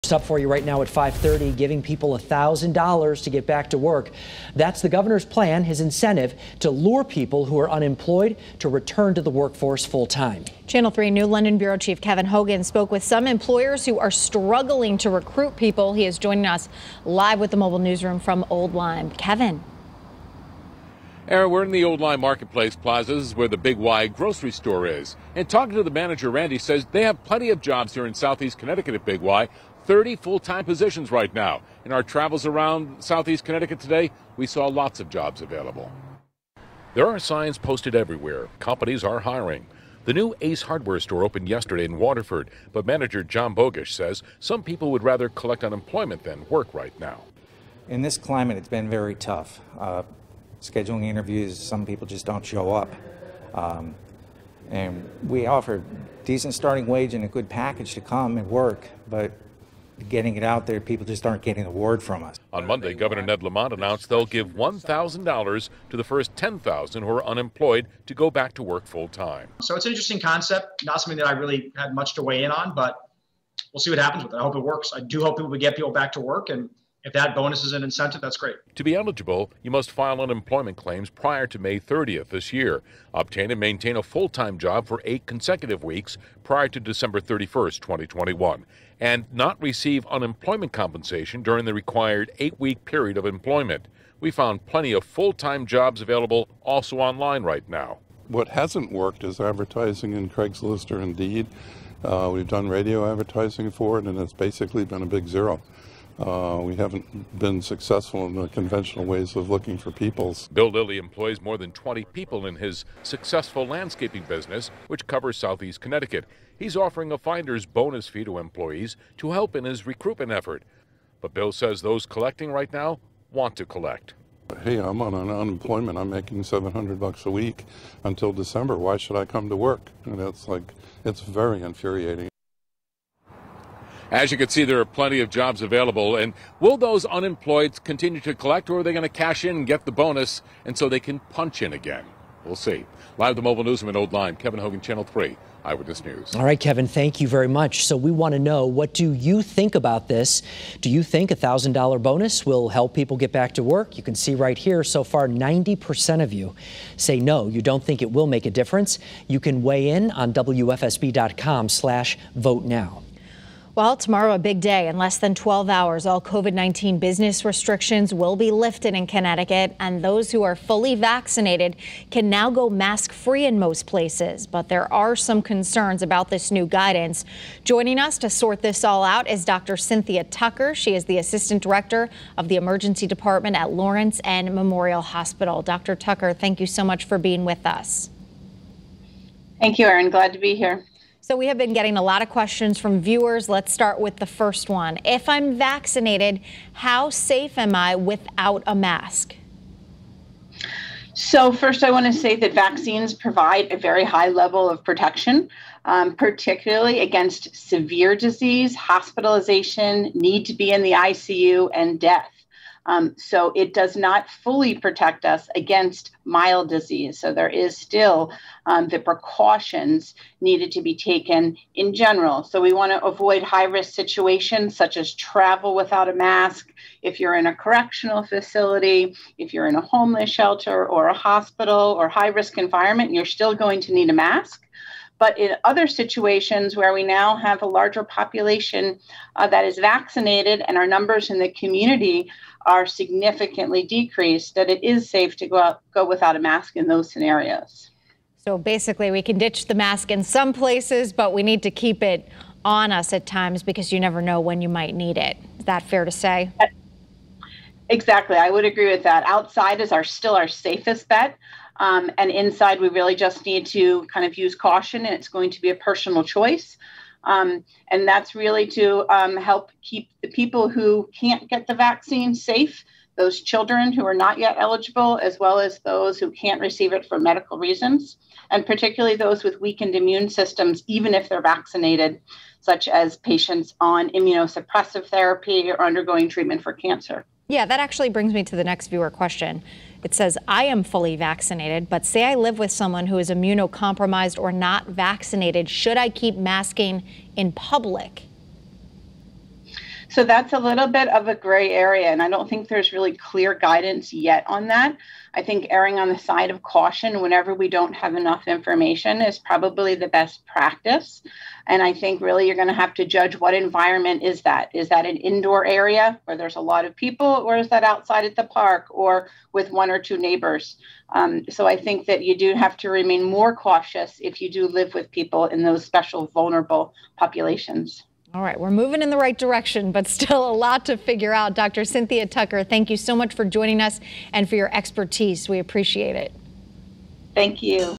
INTERVIEW: Doctor answers questions, discussing concerns over lifting mask mandate
Dr. Cynthia Tucker, the assistant director of the emergency department at Lawrence and Memorial Hospital, discusses the concerns over the new mask guidance.